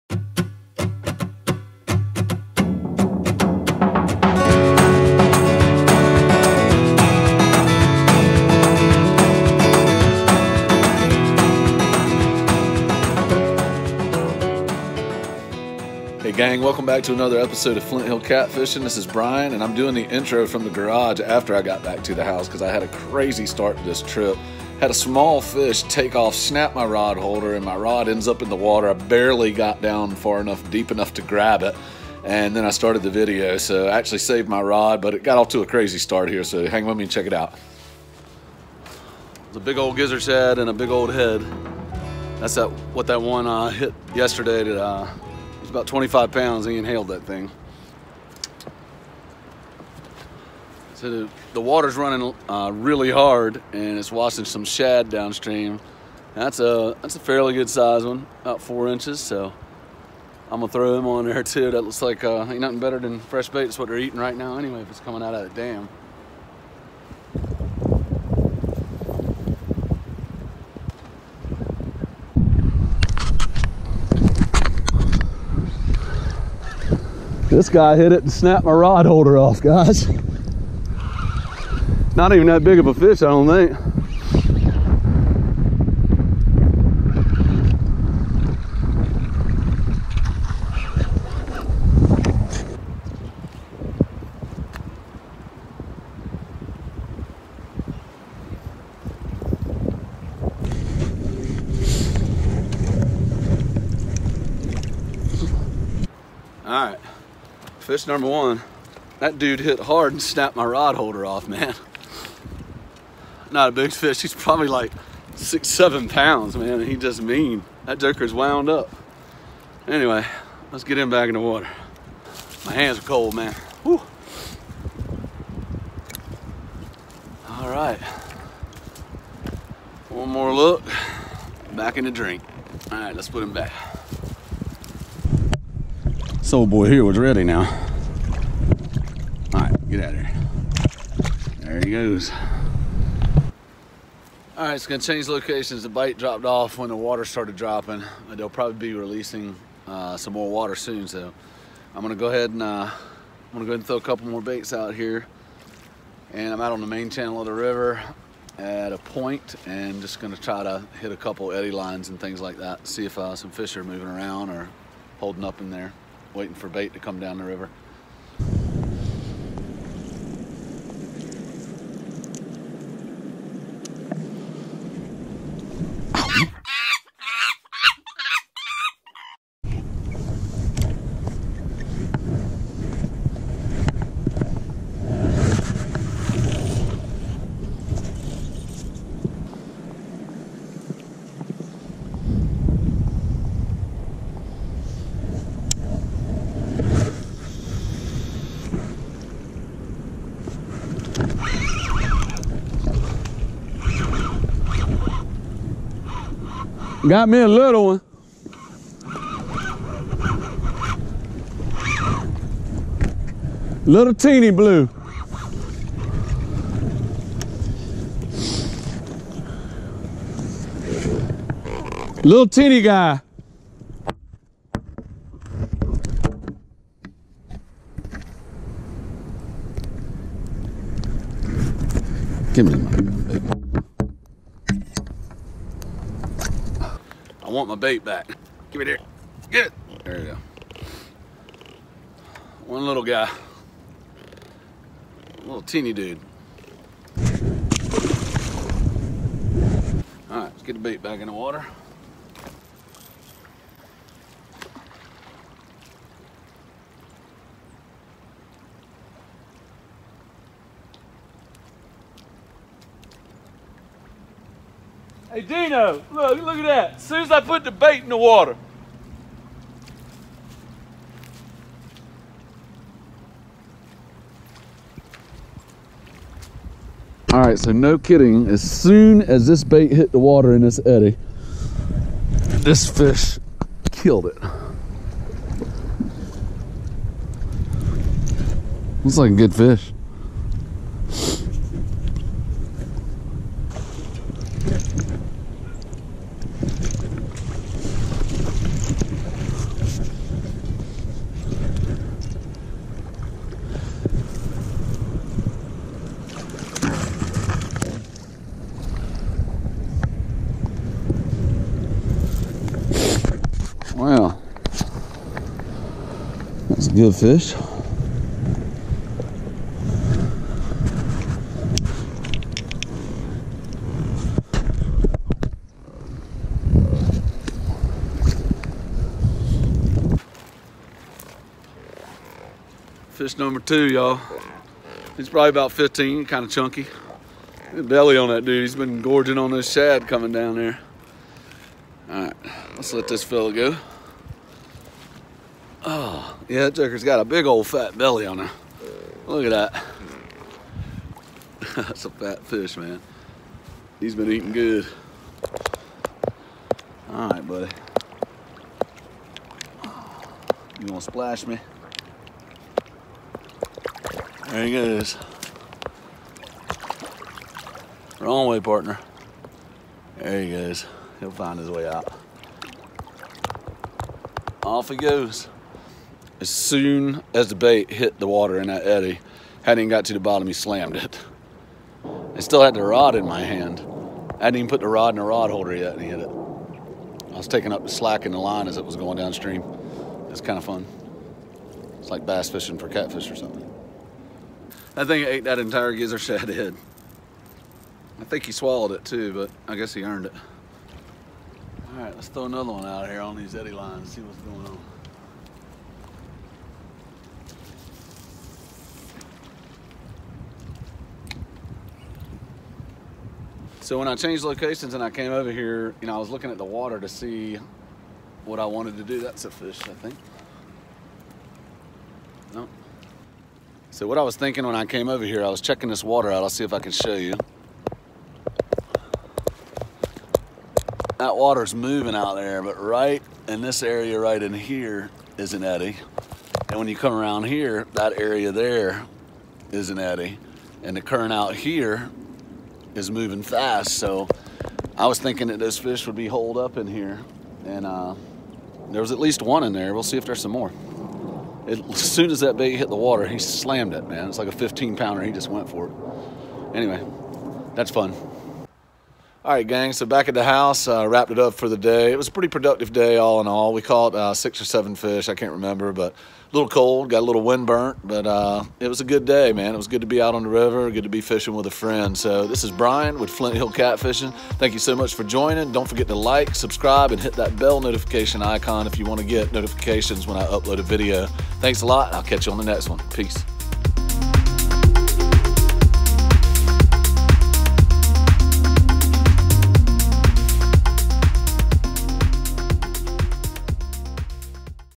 How to Catch Catfish from the Bank in Winter!
Fishing from the bank in the winter can be slow sometimes but the quality of the fish you can catch are generally much better. If you know how to catch catfish from the bank in winter, you can really have some exciting moments that won't be forgotten any time soon. You have to be willing to put in the time, use/find fresh bait and find fishing spots where fish travel through or frequent often. Here are a few moments on the river on my last trip. Enjoy!!